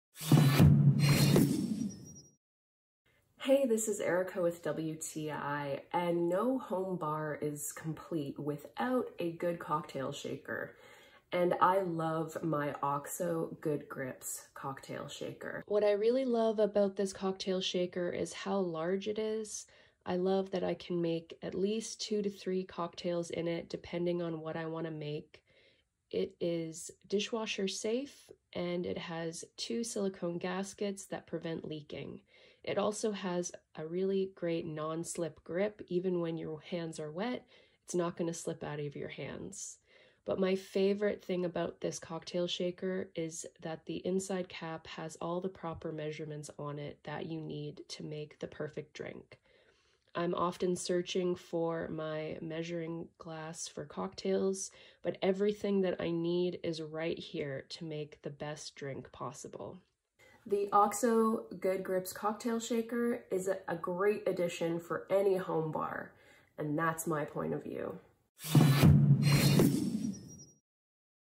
OXO Good Grips Cocktail Shakers | Our Point Of View
Check Our New Website For Amazing Deals!
(Commissionable Links)

Features two silicone gaskets for a leakproof seal when shaking
Jigger cap has measurement markings, built-in strainer
Comfortable grip points are non-slip, even when they’re wet
Dishwasher Safe single-wall construction
Holds up to 20 oz/590 mL